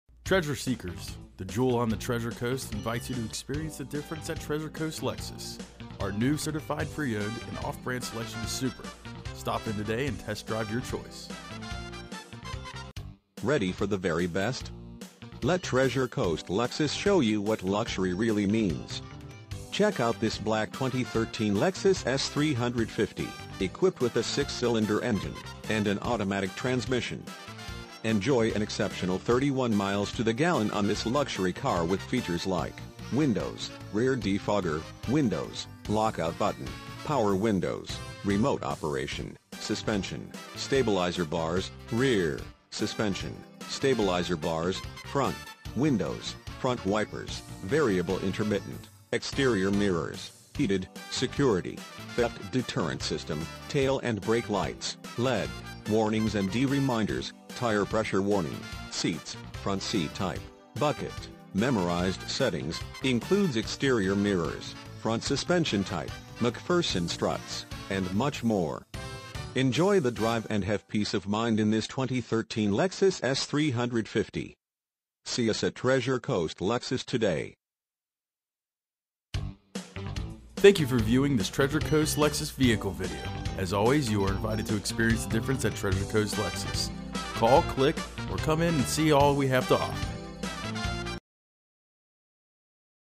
2013 Lexus ES 350 Treasure Coast Lexus Fort Pierce, FL 34982
Lexus ES Ready for the very best? Let Treasure Coast Lexus show you what luxury really means. Check out this Black Lexus Es 350 Sedan, equipped with a 6 Cylinder engine and an automatic transmission.
MILES: 24,484
VIN:JTHBK1GG9D2040498

5121 U.S. Hwy 1
Fort Pierce, Florida 34982